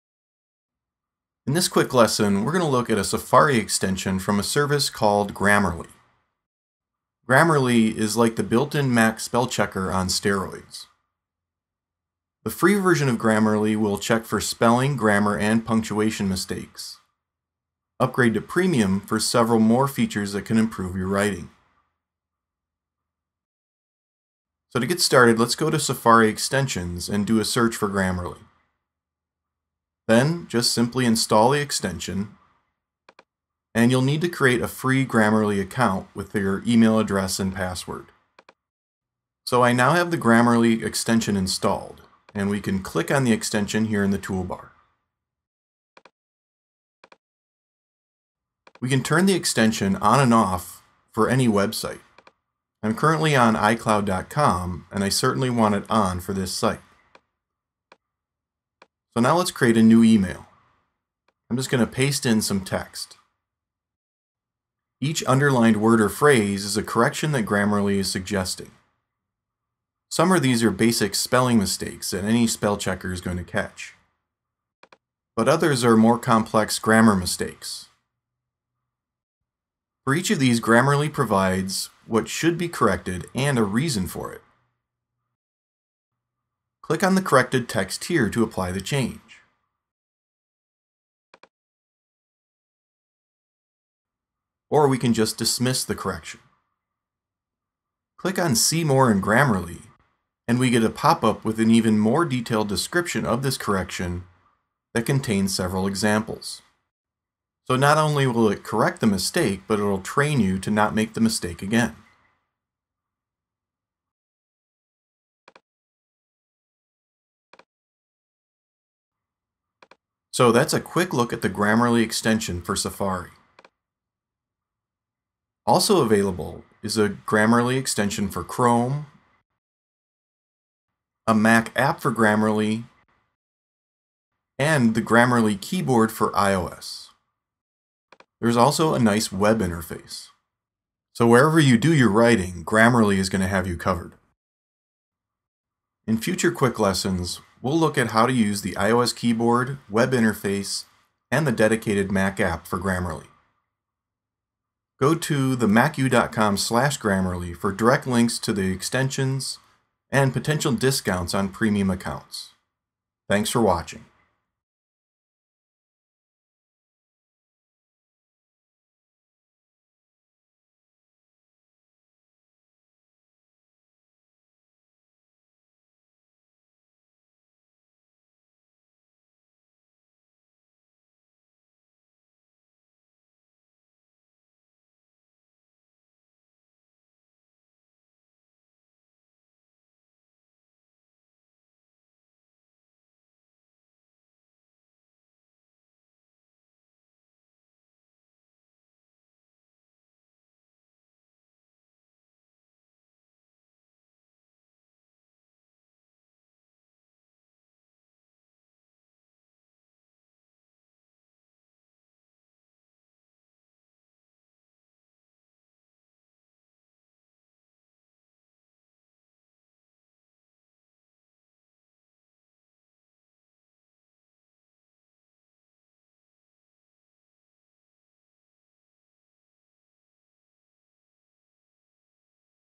Improve your writing with Grammarly: The Safari Extension
see how to use the Grammarly Safari Extension to not only correct spelling mistakes but also point out and explain grammar mistakes and changes you might want to make.